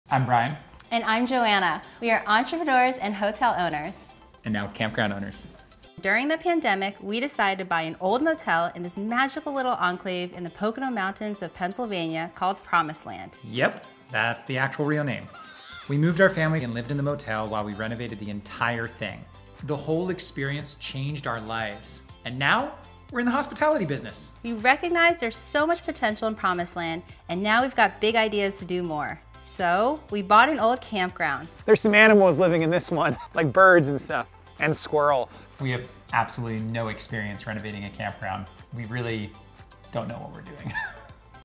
Buying a campground... with no experience
Buying a rundown campground was not in our life plan... especially a rundown one from the 1960s. Is going to be a money pit or a money maker?